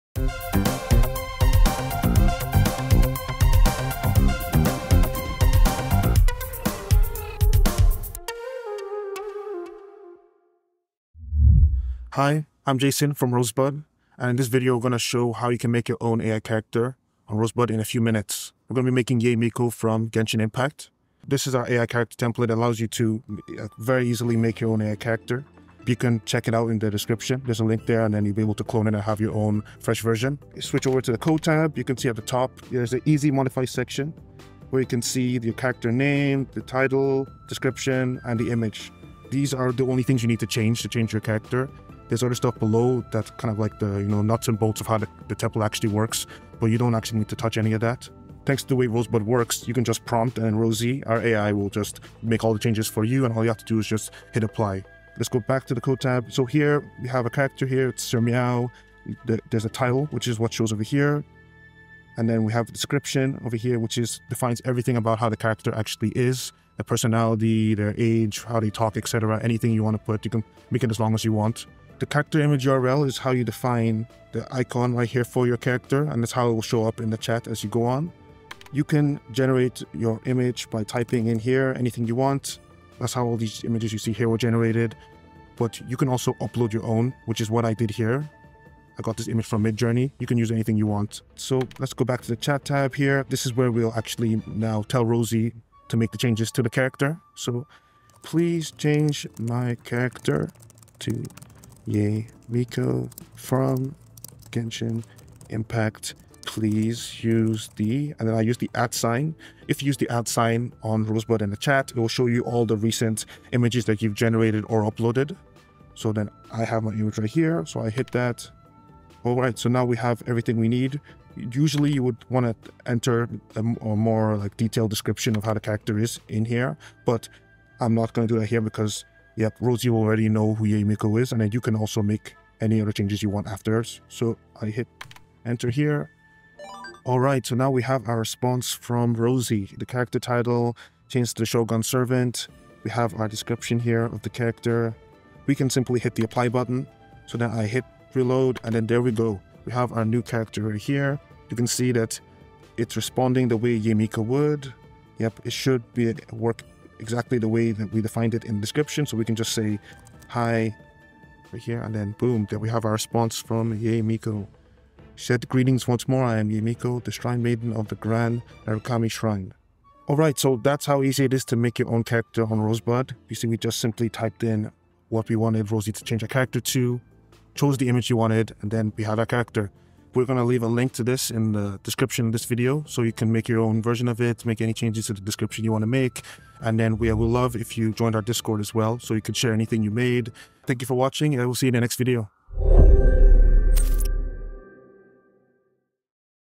Create Yae Miko AI Character in a few minutes | Rosebud AI
With Rosebud you can easily create AI characters with their own personality.

Create Games, Share and Play Instantly with Rosebud AI Gamemaker.
Go from text description to code to game.

00:00 Intro
00:10 Presentation
00:30 Exploring the code
01:20 Uploading your image
01:30 Chatting with Rosie AI to create the character
02:15 Apply changes
02:50 Join us on Discord